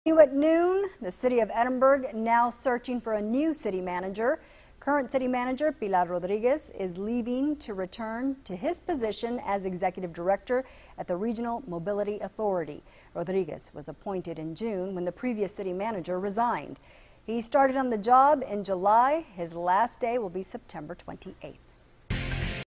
Edinburg City Manager Resigns
He will resume his position at the Hidalgo County Regional Mobility Authority.